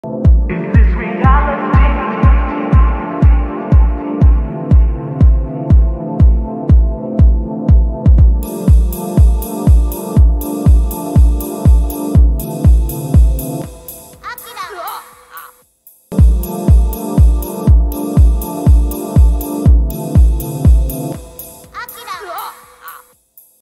cyberpunk
This took me 2 days of just messing around with shit. also i didn't use overlays it's just a custom corridor obj in element 3d and some distorted trapcode particular.

Cyberpunk amv gang gang